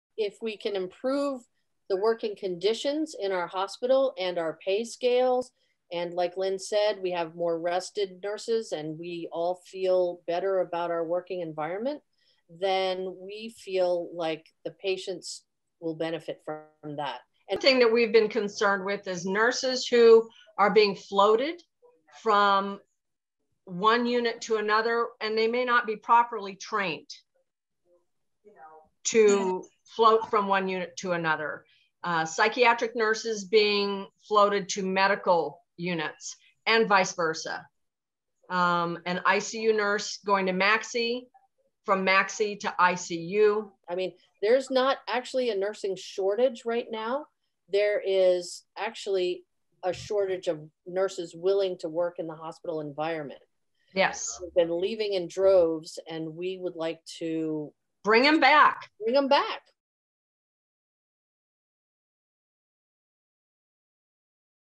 Video Now: Roger Williams nurses express concerns
The nurses at RWMC voted to unionize.